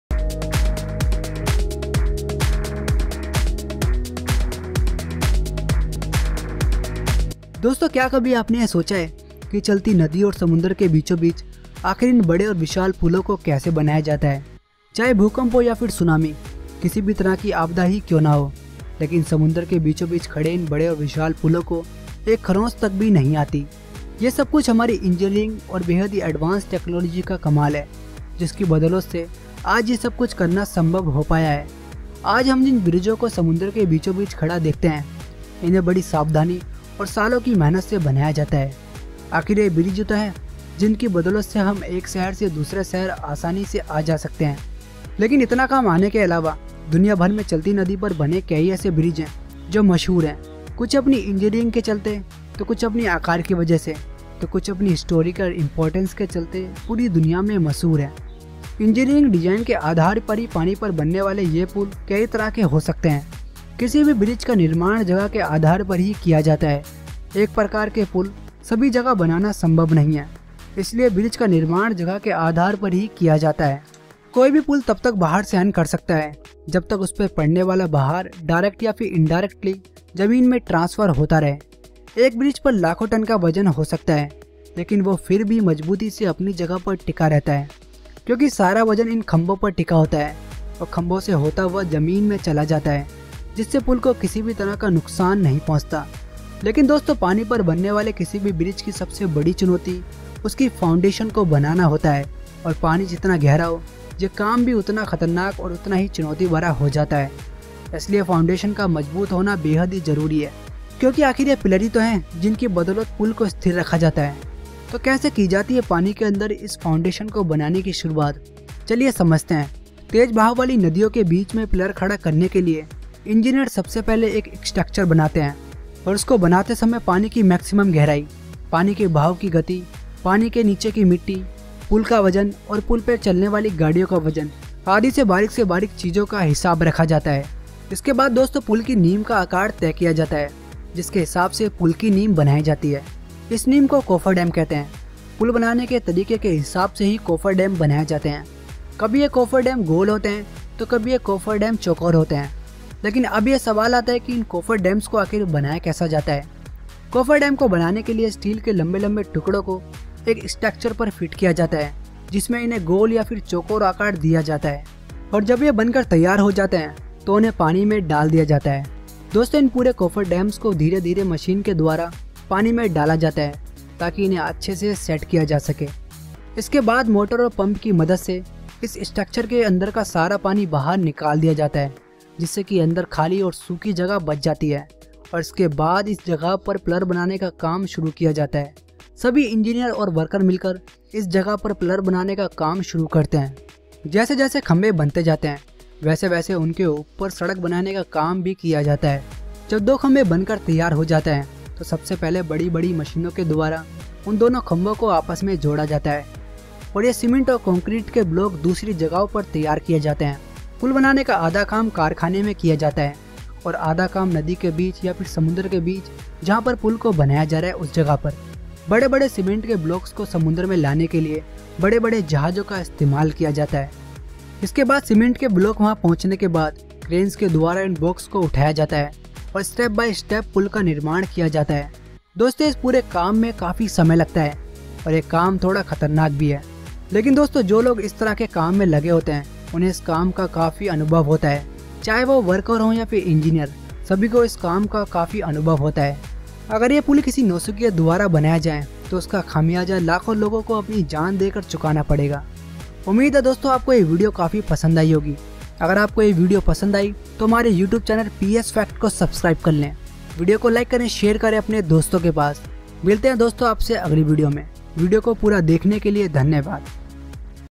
चलती नदी और समुद्र के बीचों बीच पुल कैसे बनते है / पानी के बीच मे पुल कैसे बनाए जाते है
Friends today i am going to talking about How do they build bridges over water
1 how they build bridge in sea
2 how bridges on sea? How do they build pillars for
3 how bridge engineering documentary